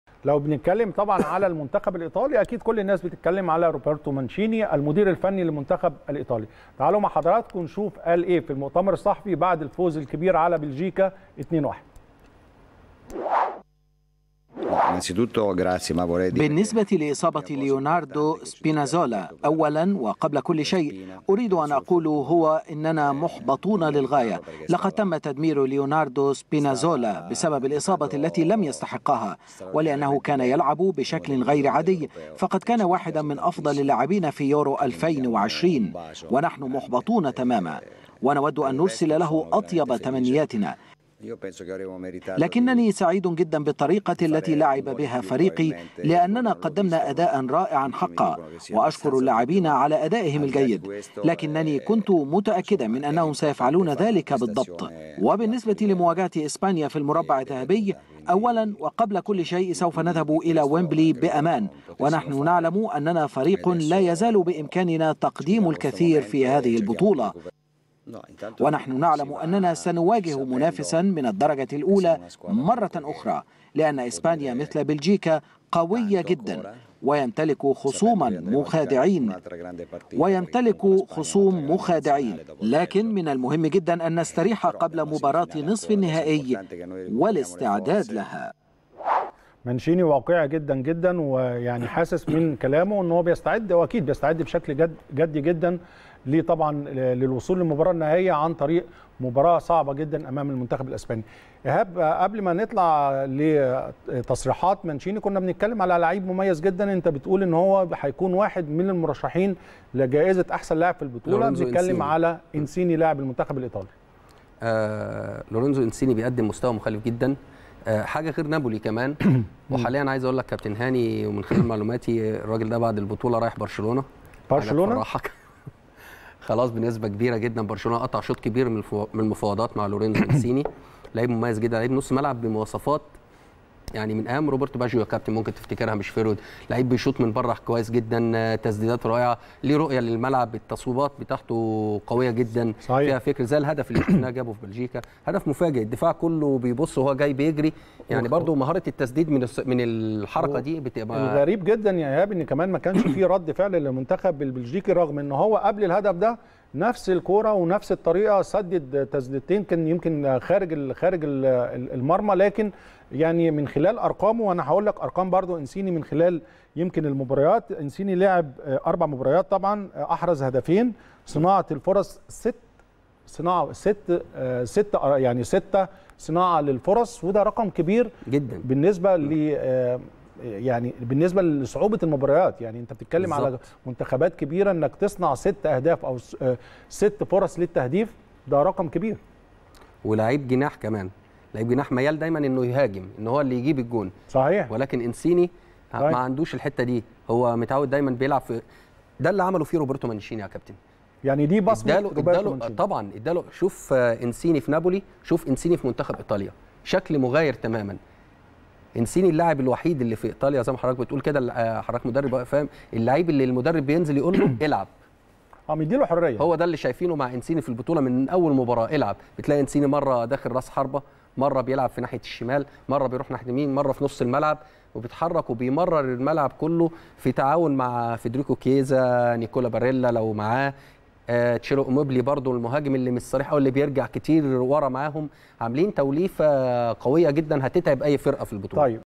الجنيدي: لورنزو إنسيني احد المرشحين لحصد جائزة افضل لاعب في يورو 2020.. وبرشلونة يسعي للتعاقد معه.
الجنيدي: لورنزو إنسيني احد المرشحين لحصد جائزة افضل لاعب في يورو 2020.. وبرشلونة يسعي للتعاقد معه | المحترف